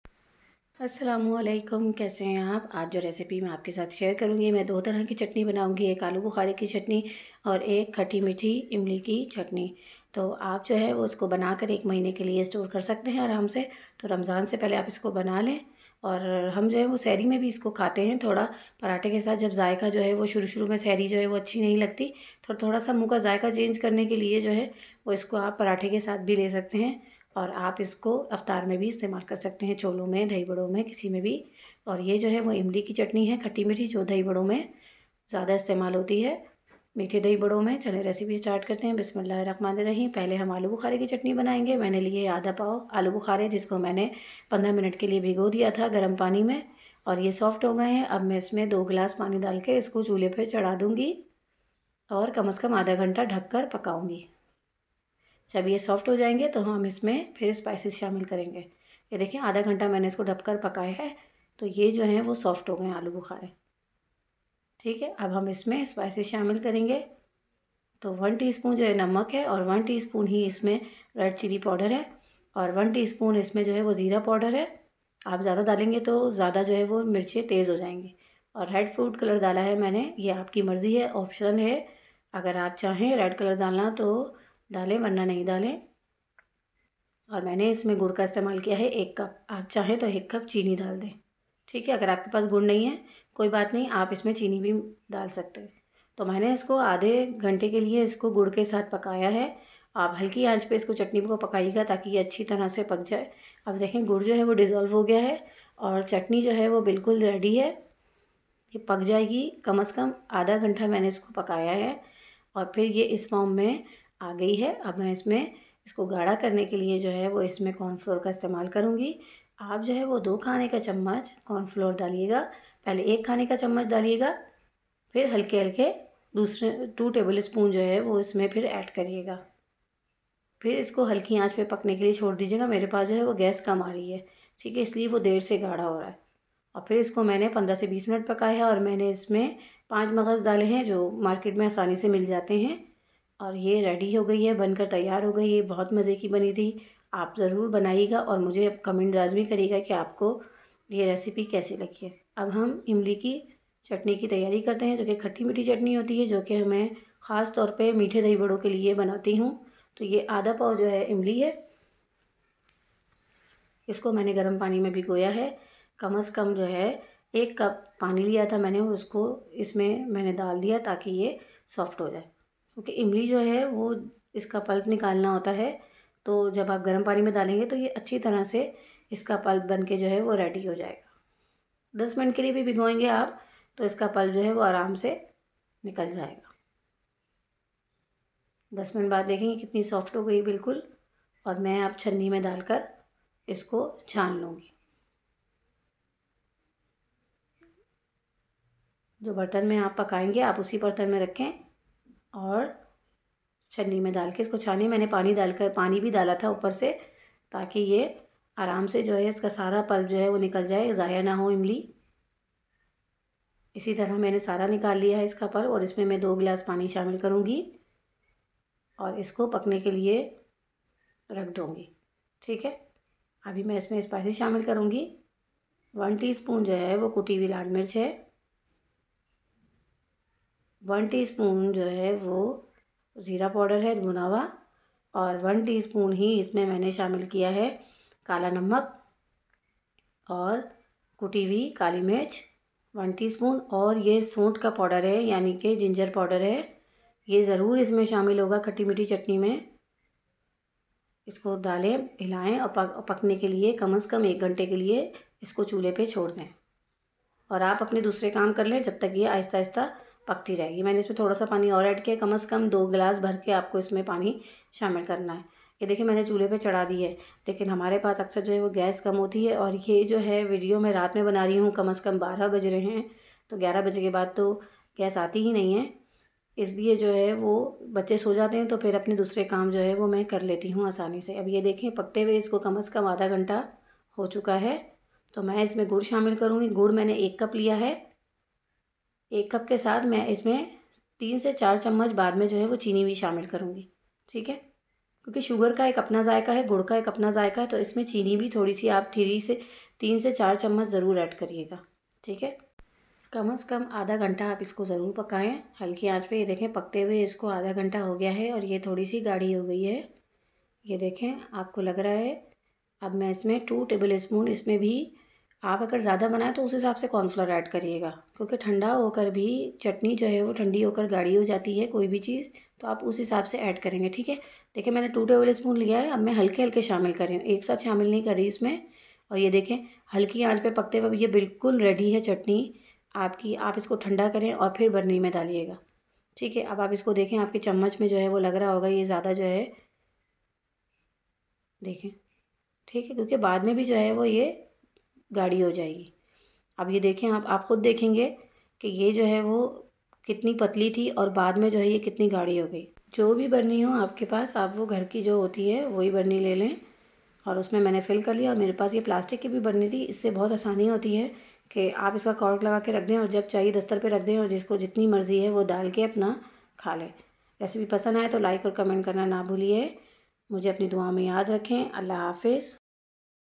Imli Ki Or Aloo Bukharay Ki Khati Methi Chatni Ramzan Special Recipe by Uraibia's Kitchen
AJ me ap ke liye Lai hu Mazedar se 2 tarha ki chatni. Khati Methi Chatni or Aloo Bukharay ki chatni Ramzan special chatni .